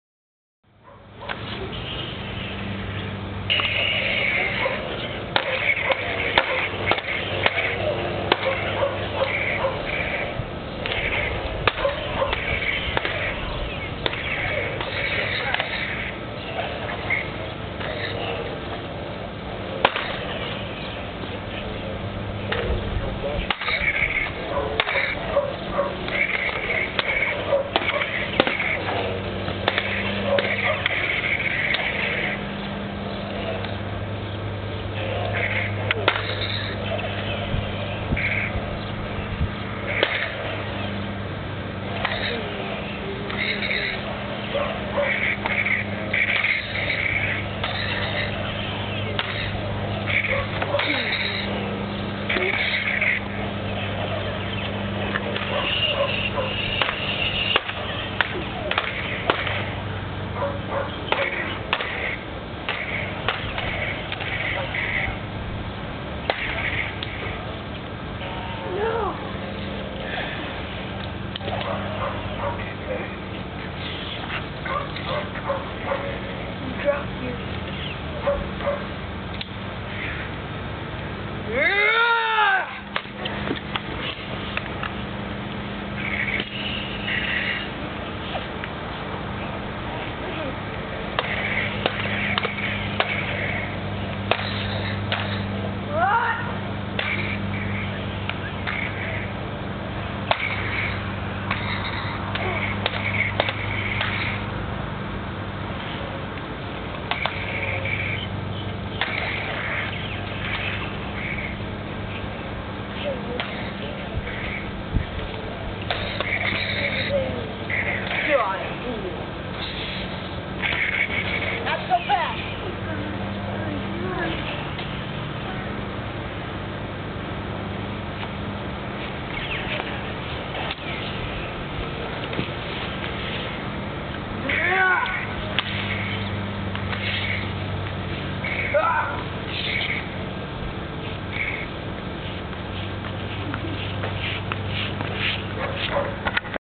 A Few Kids Lightsaber Fight
I recorded me and my friends using fake lightsabers to plan out a short little fight. It's actually kinda cool.